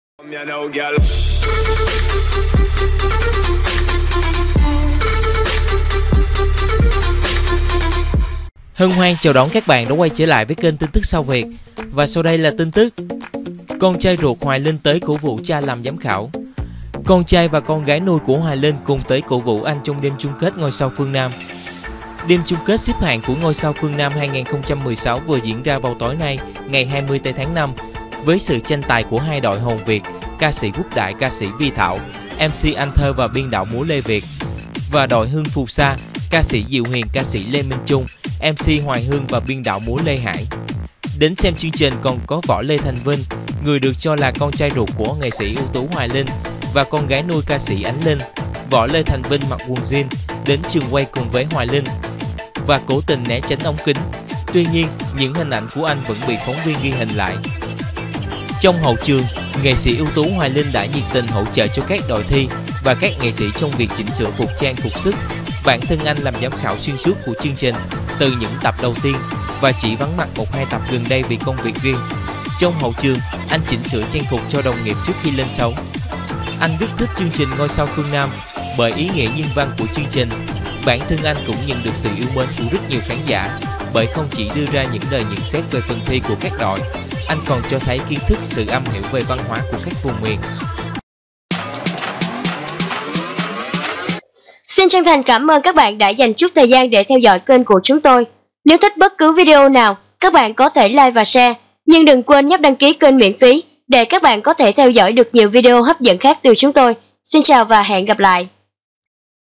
tin tuc sao viet_Con trai ruột Hoài Linh tới cổ vũ cha làm giám khảo
Đến xem chương trình có Võ Lê Thành Vinh – người được cho là con trai ruột của NSƯT Hoài Linh và con gái nuôi – ca sĩ Ánh Linh. Võ Lê Thành Vinh (mặc quần Jeans xé) đến trường quay cùng với Hoài Linh và cố tình né ống kính, tuy nhiên những hình ảnh của anh vẫn bị phóng viên ghi hình lại.